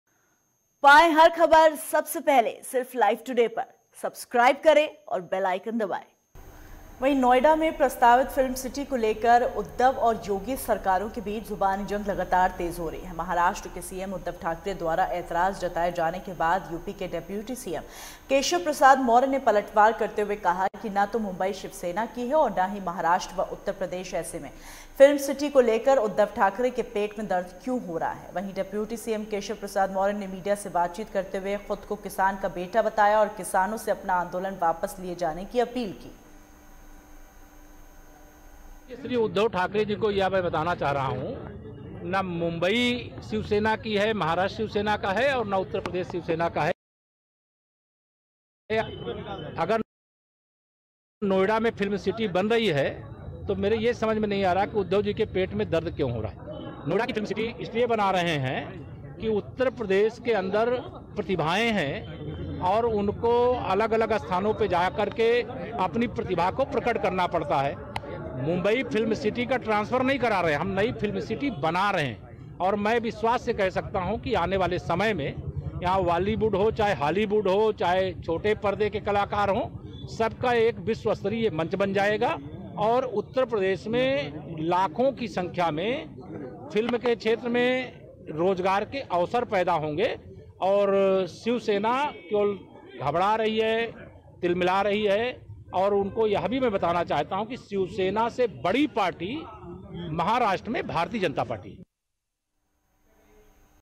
Prayagraj : UP Film City पर जुबानी जंग जारी , Deputy CM Keshav Prasad Maurya का पलटवार
हमसे जुड़े रहने के लिए हमें फॉलो करें